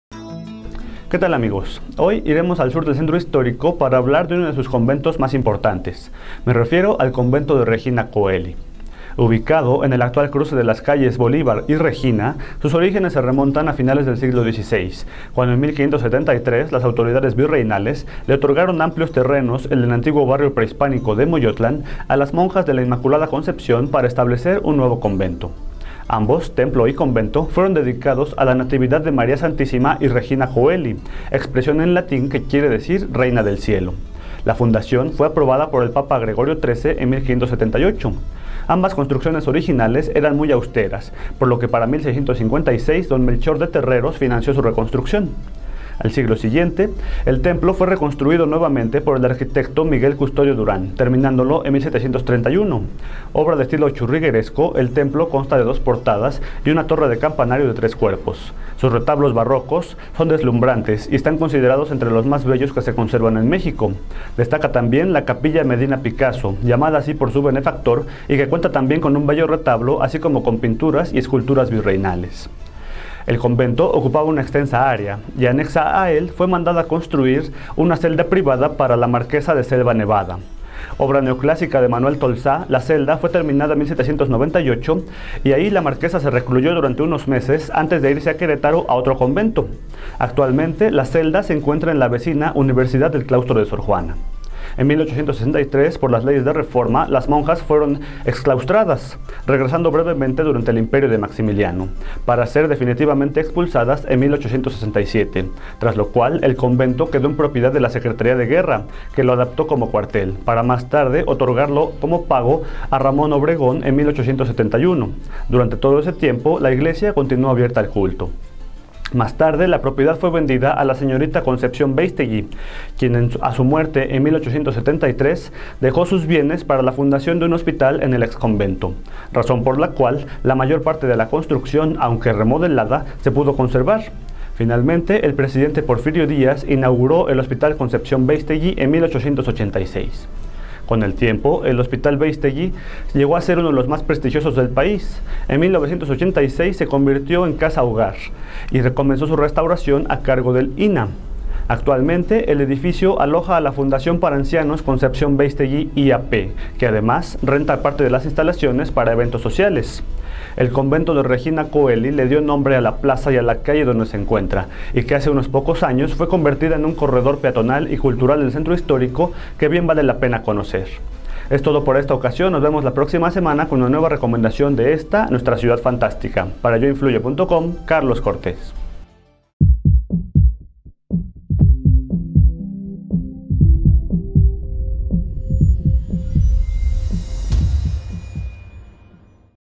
La Reina del Cielo en el Centro
Comentario Carlos J. Cortés / Fundado a finales del siglo XVI por monjas del Convento de la Concepción, y remodelado a lo largo de los siglos; el convento de Regina Coeli se convirtió en uno de los más bellos de la Nueva España, sobre todo su templo; además, tuvo una celda privada, construida por Manuel Tolsá. Finalmente en el siglo XIX terminó convirtiéndose en uno de los hospitales privados más prestigiados del país, fundado por Concepción Béistegui. Conoce más.